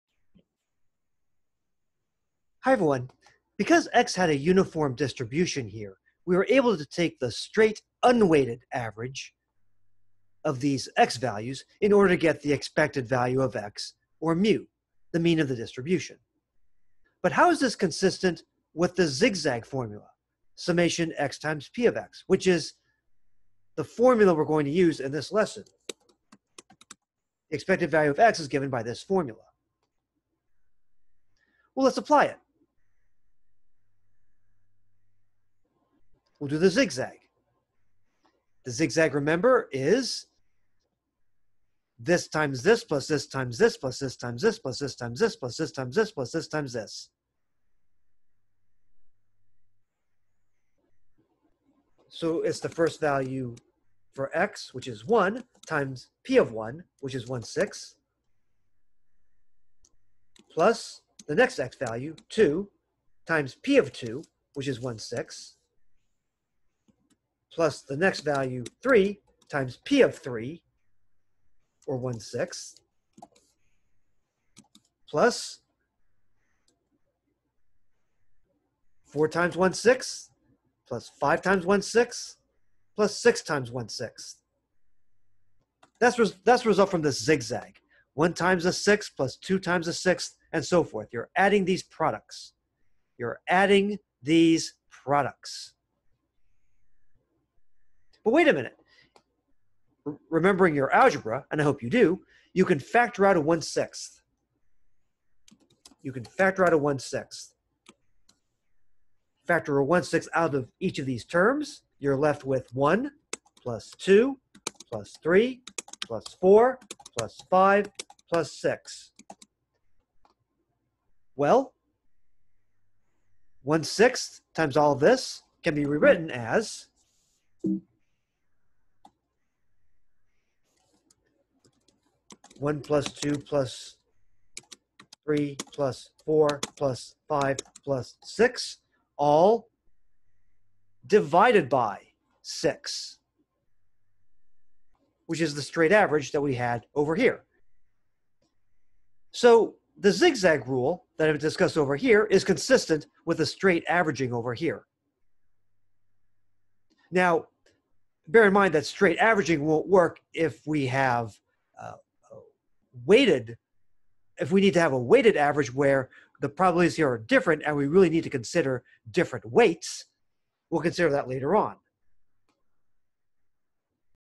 Math 119, Day 10, Part 3 (Optional if You Watch the Later Videos)
Lesson 16: Expected Value: Applying the “Zigzag Rule” (this is also done in later videos).